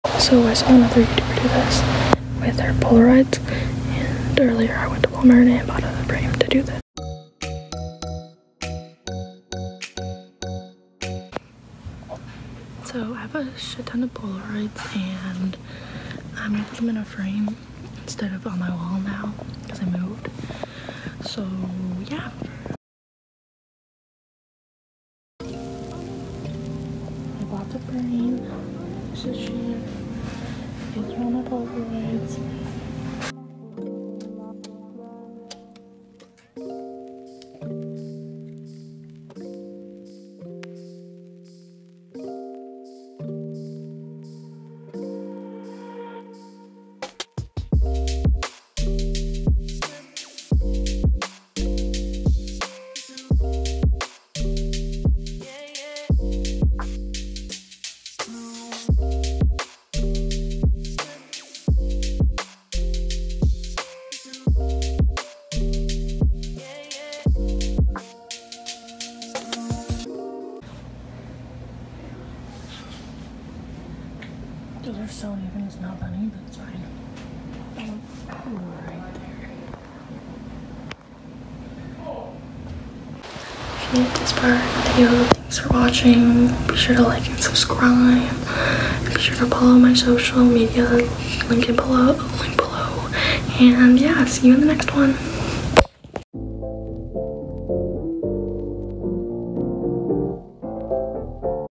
quarantine crafts
hey guys welcome back to my channel. i just recently moved and my Polaroids have been sitting in a box so i thought i do something to fix that so hope u enjoy also be sure to give me a follow on my socials